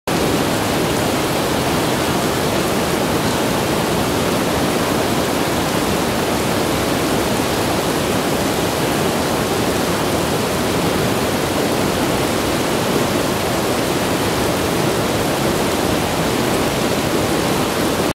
Eagle Creek.MTS
Eagle Creek trail, Columbia Gorge, 1 hour east of Portland, Oregon. Shot with Canon Vixia HF S200 at 1080P, on tripod (in the middle of the creek). May 14, 2012.